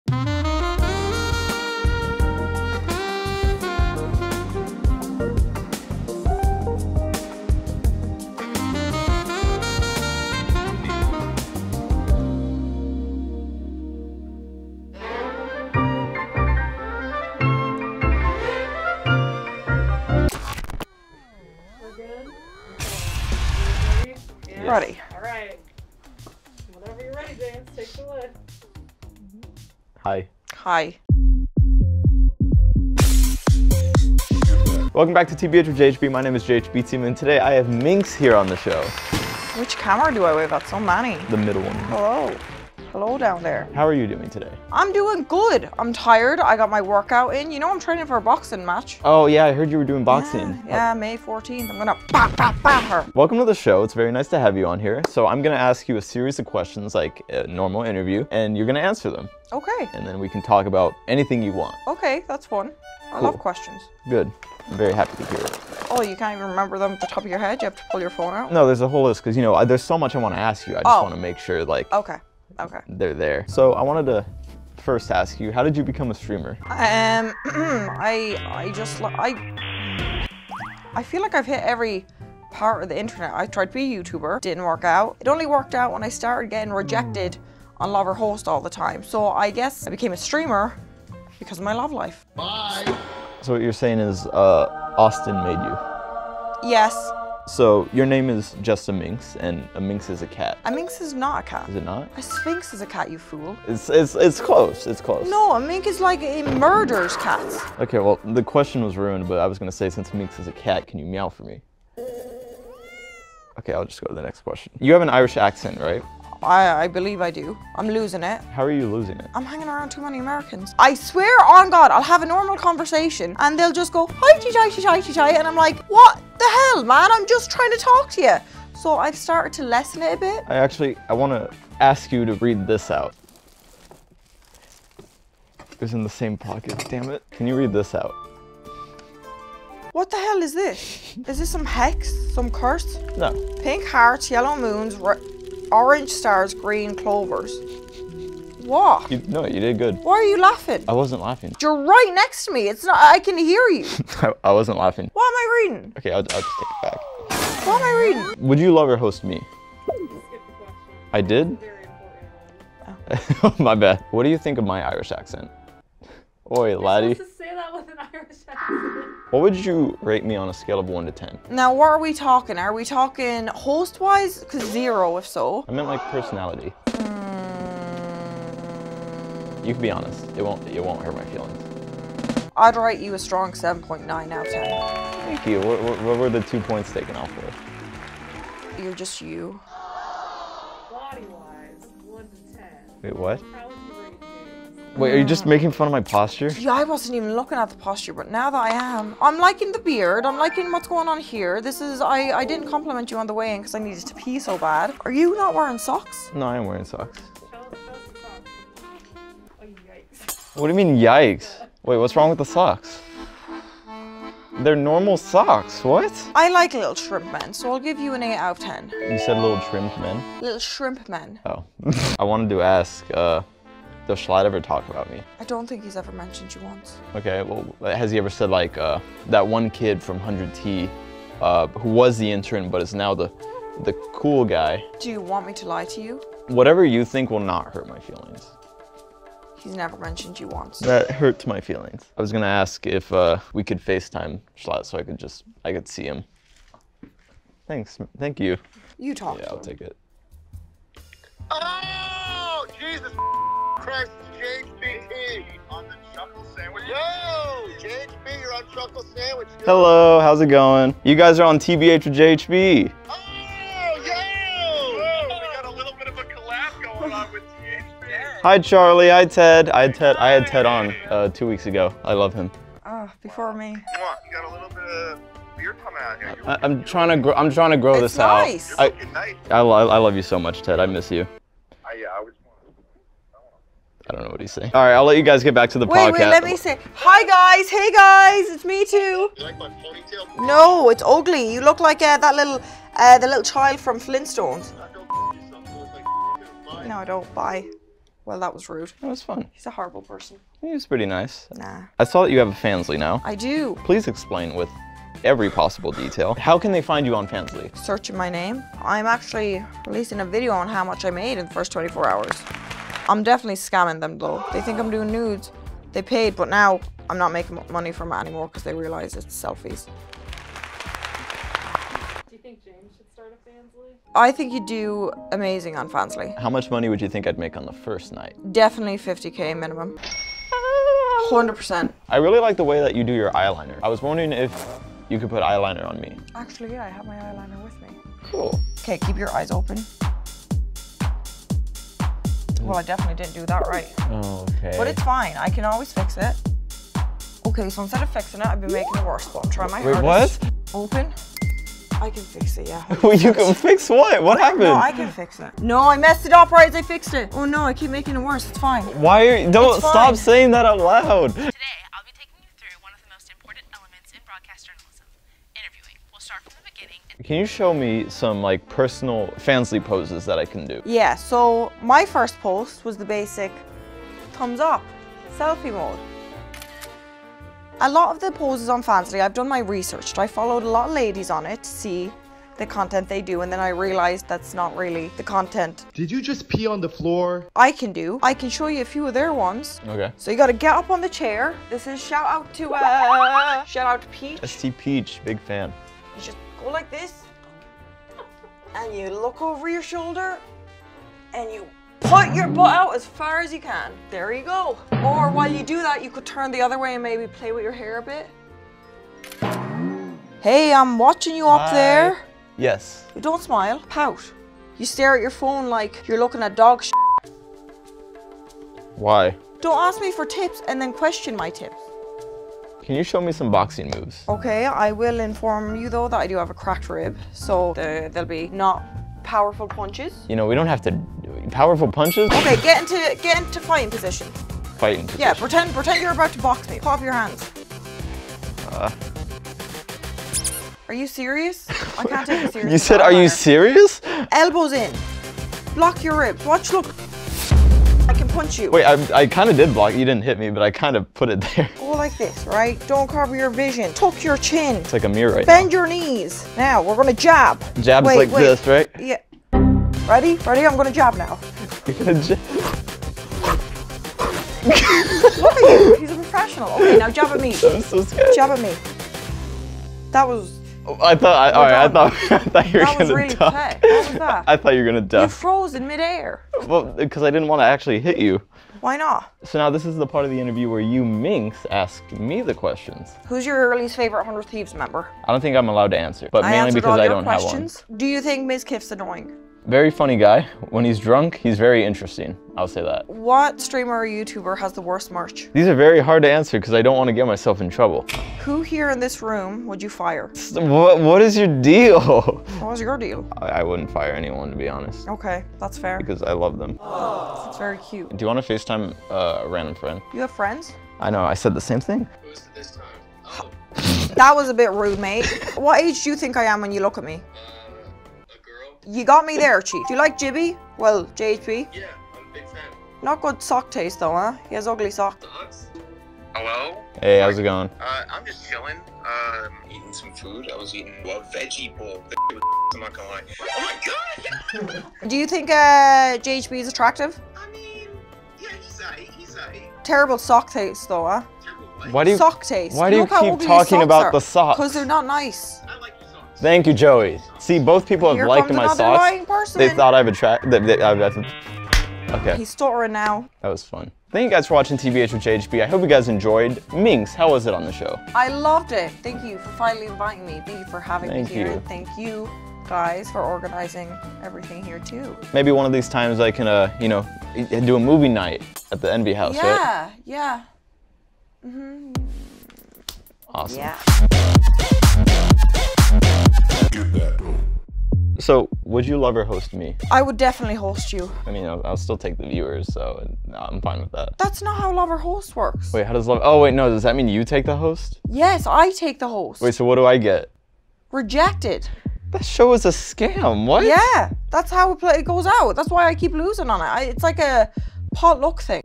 JHB FIGHTS JUSTA MINX!? (ft. jschlatt)
Welcome back to another episode of TBH w/ JHB! Today, JHB interviews (and fights???) Justa Minx, along with a special guest appearance from jschlatt! So uh... full disclosure here, I'm giving Autumn a break from writing the usual glorious descriptions... so you are stuck with muah (Gustavo the YT Manager :D). I could go on and on and tell you what crazy things James does during this episode, but I want to make sure you all watch it ALL THE WAY THROUGH. So instead, i'll leave a few words here that have to do with the episode and you need to leave a comment with the timecode when you find what each word(s) refers to. I'll let you know if you got it right after! Okay? Ok. (if you are still reading anyway). Okay, Here goes: lucky charms; flintstones; pretty jhb; whiff; will smith; sock taste; 0.